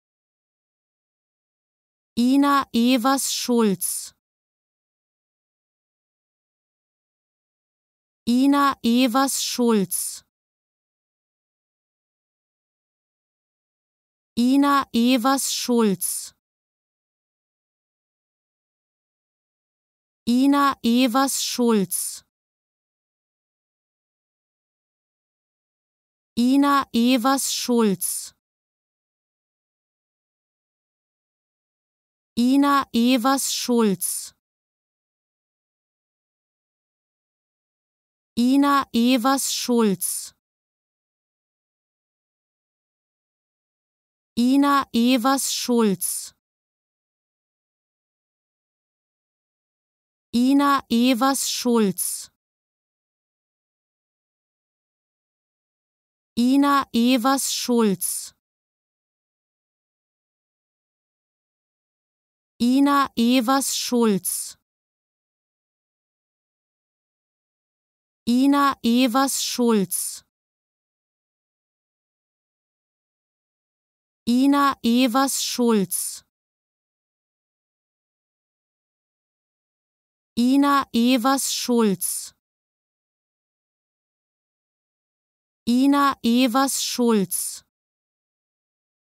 How to Pronounce ''Ina Ewers-Schultz'' Correctly in German
Learn how to say ''Ina Ewers-Schultz'' in German with this short pronunciation tutorial.